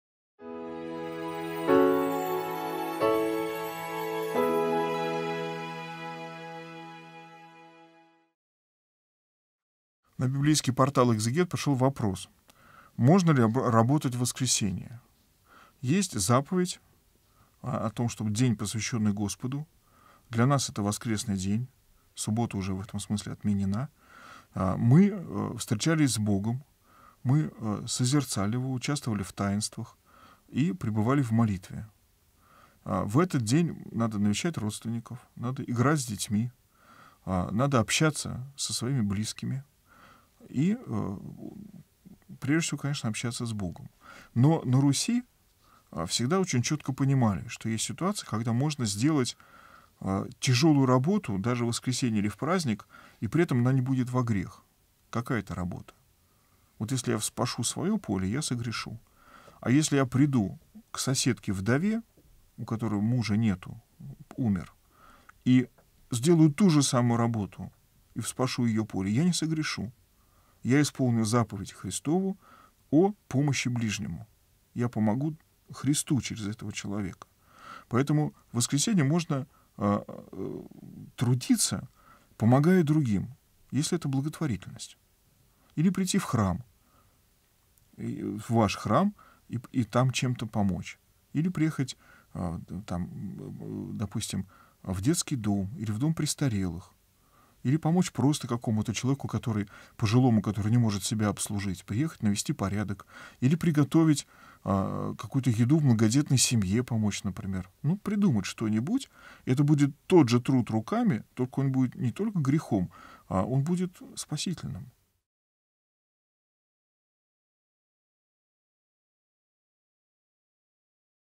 Можно ли работать в воскресенье ? Протоиерей Феодор Бородин
В сегодняшнем видео отец Феодор Бородин рассказывает о том, можно ли работать в воскресенье.

🔸 День субботний.
🔸 Участие в Церковных Таинствах.
🔸 Помощь ближнему.
🔸 Служба в храме.
🔸 Добродетель.